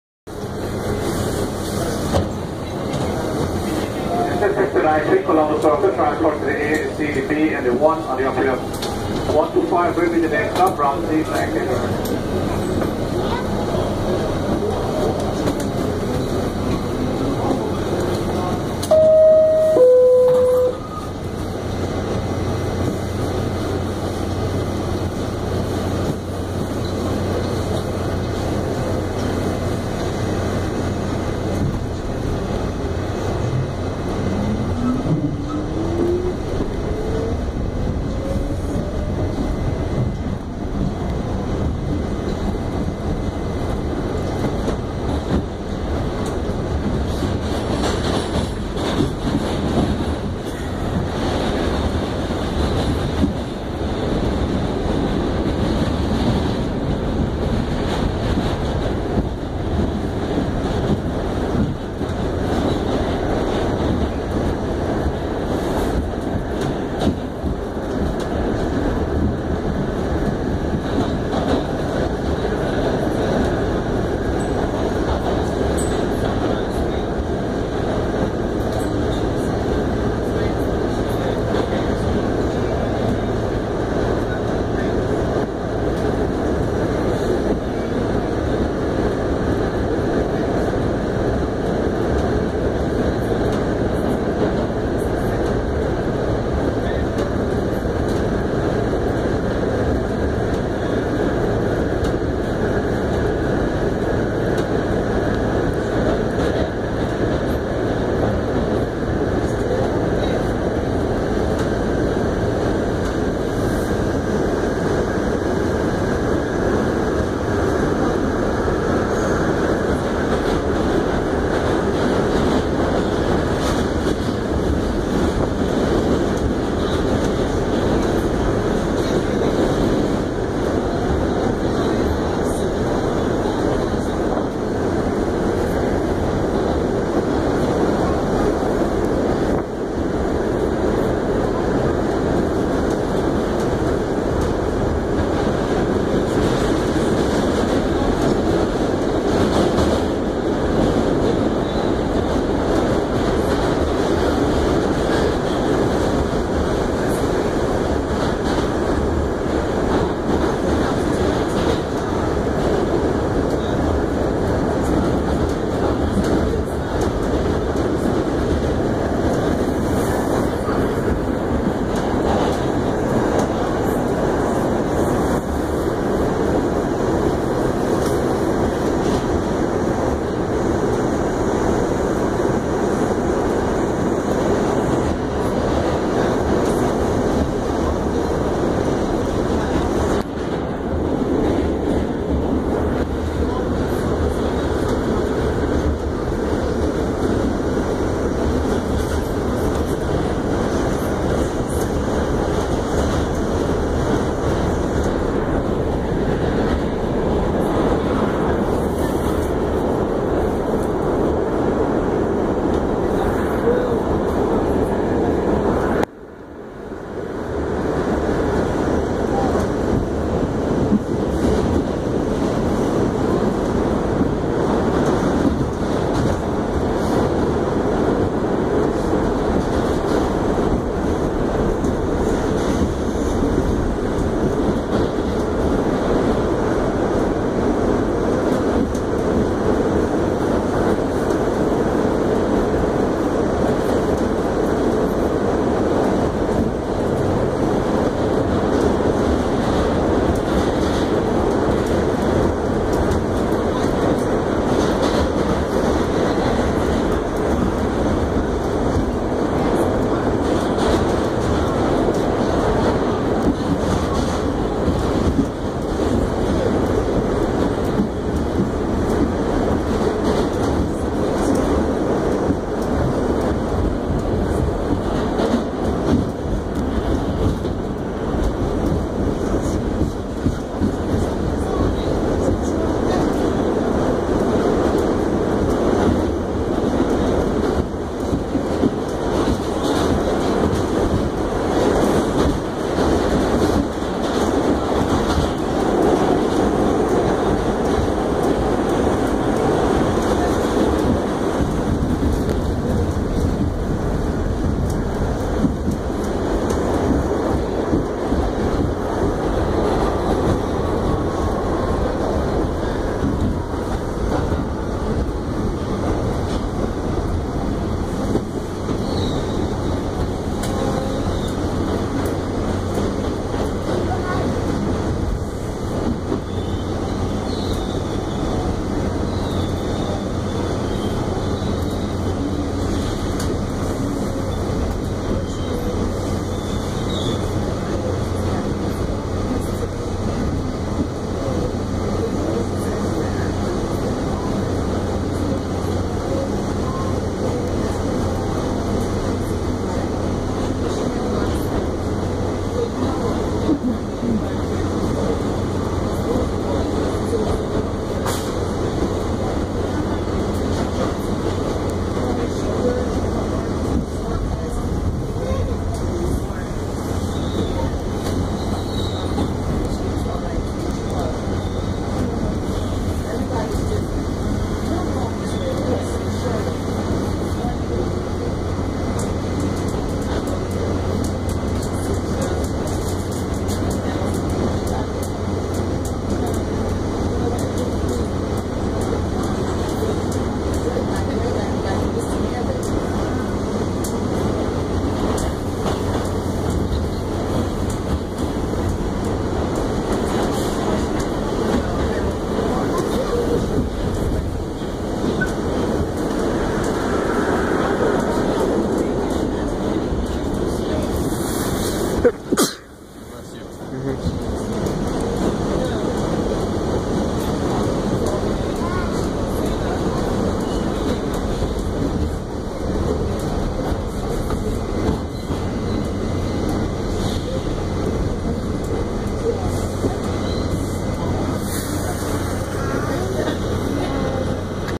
R68 (D) train ride 59th Street To 125th Street!
On Board A Bronx Bound R68 (D) Express Train Leaving 59th Street Skipping 72nd Street 81st Street 86th Street 96th Street 103rd Street 110th Street 116th Street And Stopped Due To Train Traffic And I Decided To Stop There!
Sorry For My Cough I Was Very Sick The Day This Was Recorded.
IND 6th Avenue Line
Video Taken November 13th 2014!